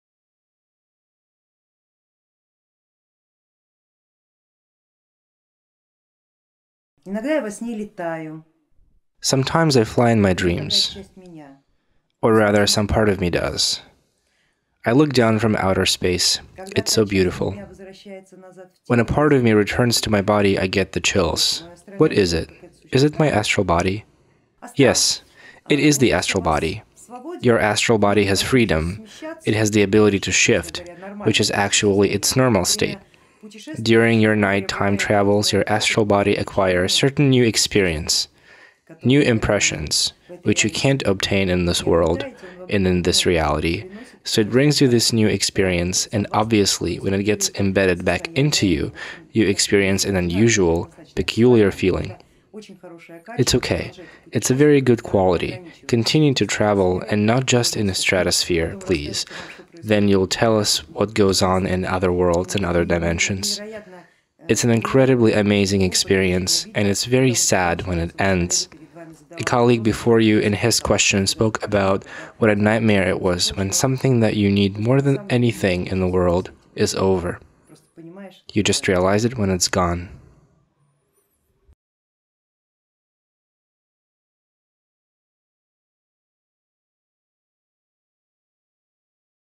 Astral Travel
- Sometimes I fly in my dreams , or rather some part of me does, I look down from outer space, it's so beautiful. When a part of me returns to my body, I get chills.  What is it? Is it my Astral body?
- Yes, it is  the Astral body. Your Astral body has  freedom, the ability to shift, which is actually a normal state. During your night time travels your Astral body acquires certain  new experience, new impressions which you can’t obtain from this world, in this reality, so it brings you this new experience and, obviously, when it gets embedded back into you, you experience an unusual, peculiar  feeling.